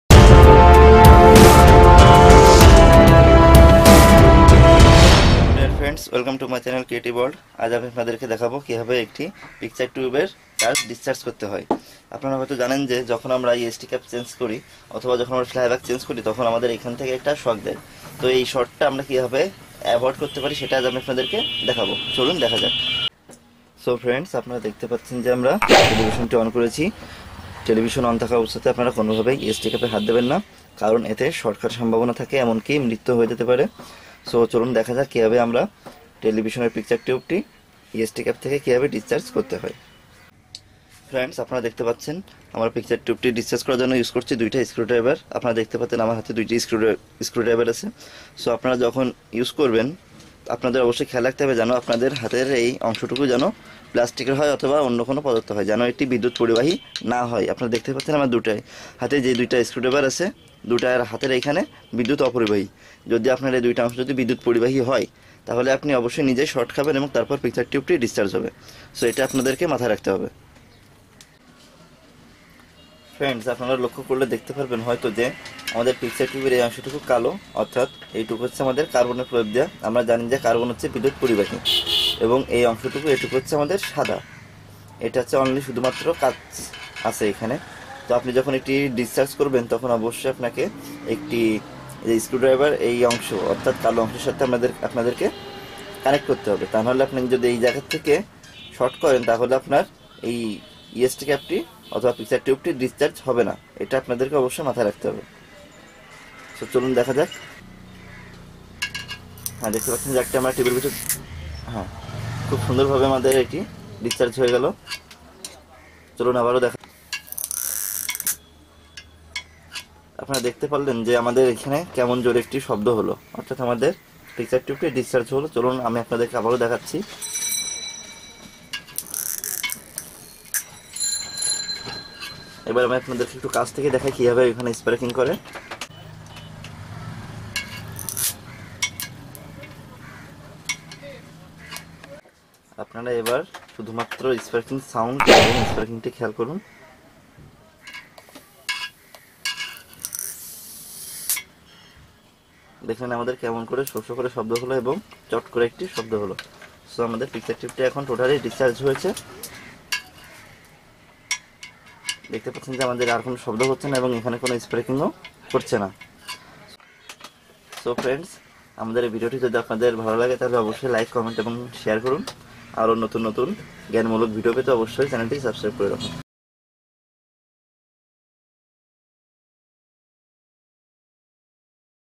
How To Discharge Picture Tube (For The Beginners) - Bengali Tutorial
Discharging Picture Tube Is A Basic Knowledge Of Electronics. To Repair Picture Tube You Might Need To Know This.
We Try Our Best to Realize You.